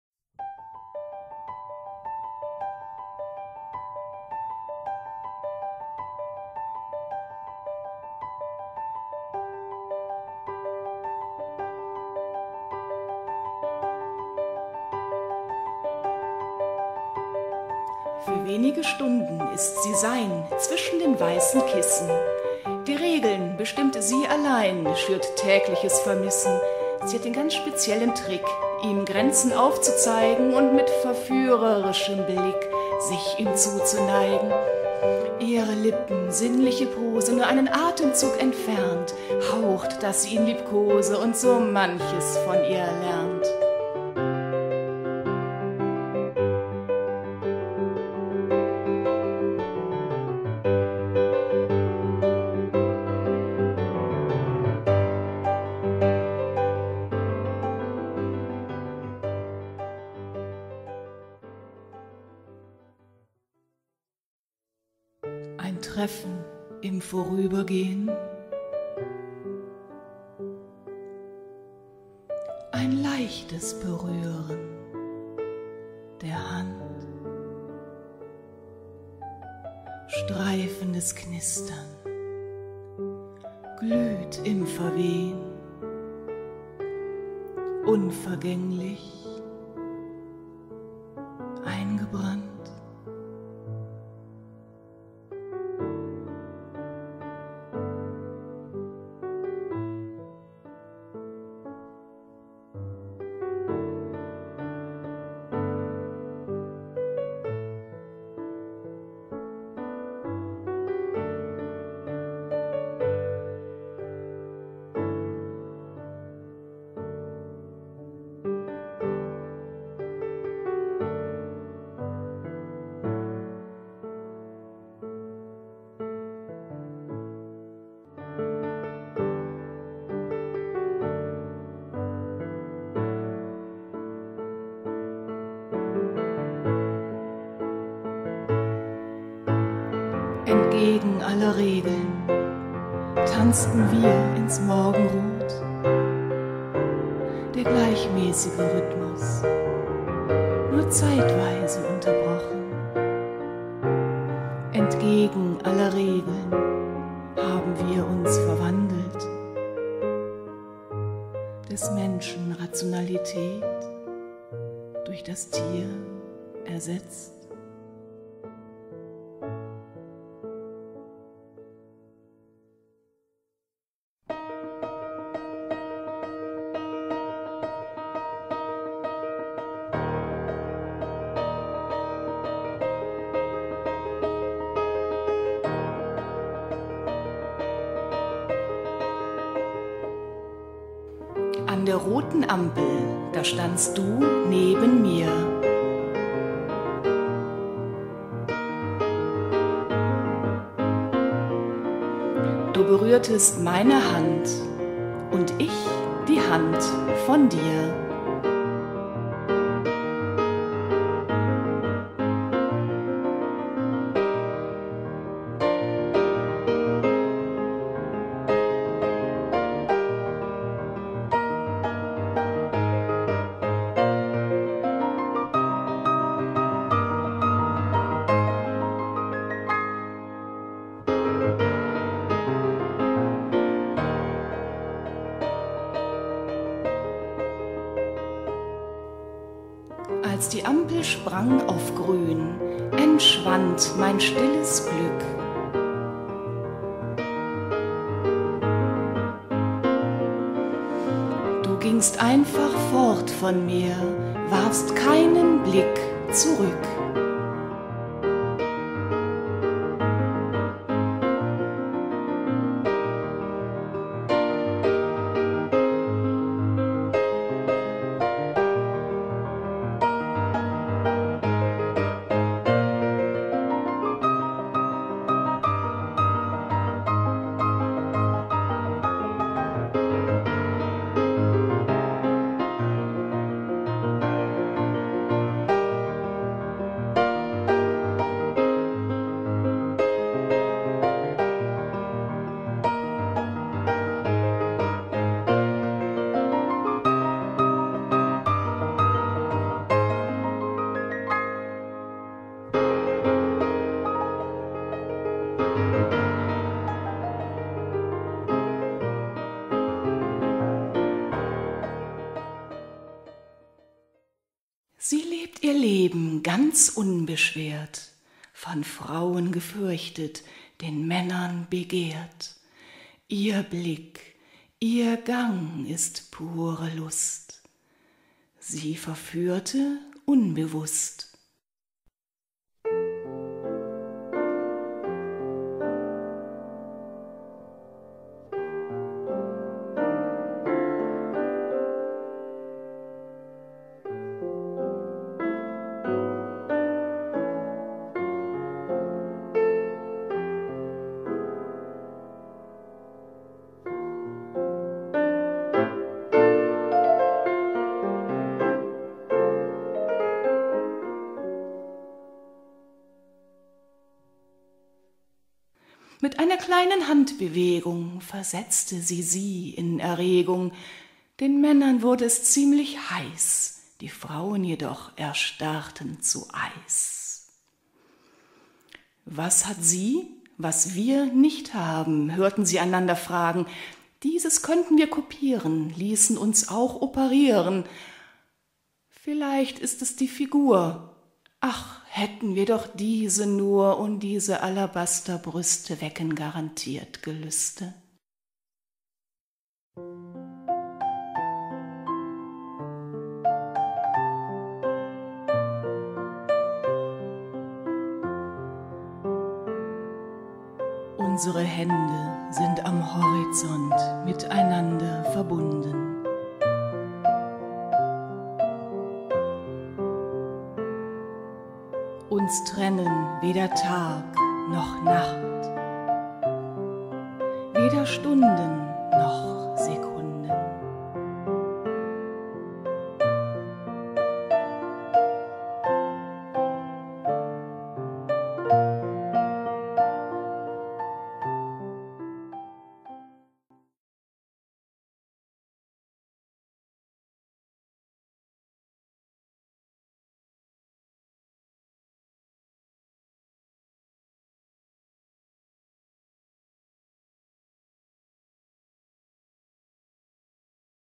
Halbmondzeiten Sinnliche AugenBlicke
Ausschnitte aus dem Programm "Sinnliche AugenBlicke". Demnächst live mit Alexander Albrandt am Piano!
Text: Geschrieben und gesprochen Christa Lehmann, Kompositionen: J. Schmidt, R. Wrasse, H. Kleine-Horst , R. Spielberg, Piano: Hansmartin Kleine-Horst, Fotos: U. Theuer, I. Caspari, D. Lehmann